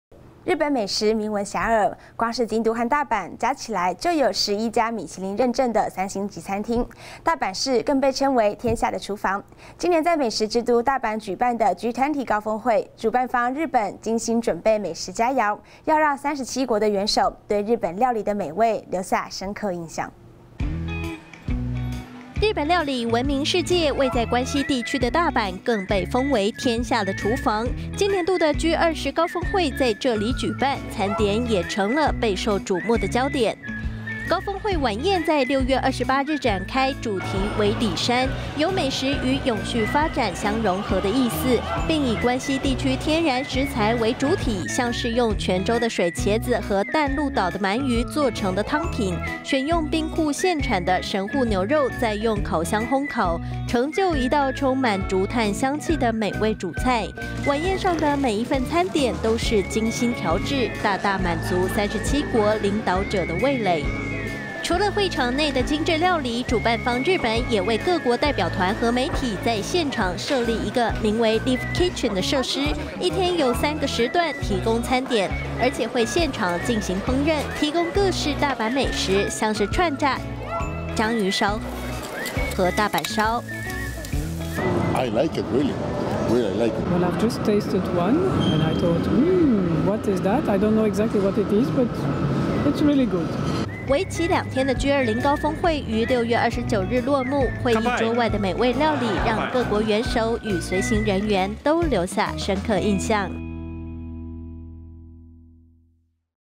日本美食 打動G20高峰會37國元首味蕾【大千世界】神戶牛肉
【新唐人訊】日本美食名聞遐邇，光是京都和大阪，加起來就有11家米其林認證的三星餐廳，大阪市更被稱為「天下的廚房」。今年在美食之都大阪舉辦的G20高峰會，主辦方日本精心準備美食佳餚，要讓37國元首對日本料理的美味，留下深刻印象。

日本料理聞名世界，位在關西地區的大阪更被封為「天下的廚房」，今年度的G20高峰會在這裡舉辦，餐點也成了備受矚目的焦點。

高峰會晚宴在6月28日展開，主題為「里山」，有美食與永續發展相融合的意思，並以關西地區天然食材為主體，像是用泉州的水茄子和淡路島的鰻魚做成的湯品，選用兵庫縣產的神戶牛肉，再用烤箱烘烤，成就一道充滿竹炭香氣的美味主菜，晚宴上的每一份餐點都是精心調製，大大滿足37國領導者的味蕾。

除了會場內的精緻料理，主辦方日本也為各國代表團和媒體在現場設立一個名為Live Kitchen的設施，一天有三個時段提供餐點，而且會現場進行烹飪，提供各式大阪美食，像是串炸(Kushikatsu)、章魚燒和大阪燒。

新聞記者 Rodrigo Mendoza：「我很喜歡，真的真的很喜歡。」

歐洲理事會成員 Isabelle Brusselmans：「我吃了一口，我覺得，『嗯...那是什麼？』我不知道這到底是什麼食物，不過它真的很好吃。」

為期兩天的G20高峰會於6月29日落幕，會議桌外的美味料理，讓各國元首與隨行人員都留下深刻印象。

新唐人亞太電視 Yolin 綜合報導